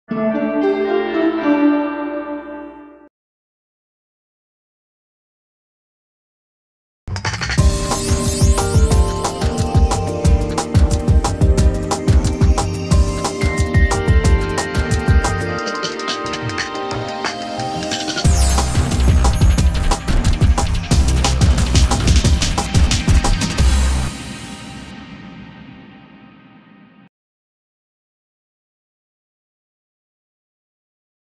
SPOT NASIM PUBLICIDAD
Spot para transmisión televisiva. Nasim publicidad es la agencia de medios mas reconocida de la Riviera Maya. Trabajamos en toda la Riviera maya, con calidad HDV High Definition.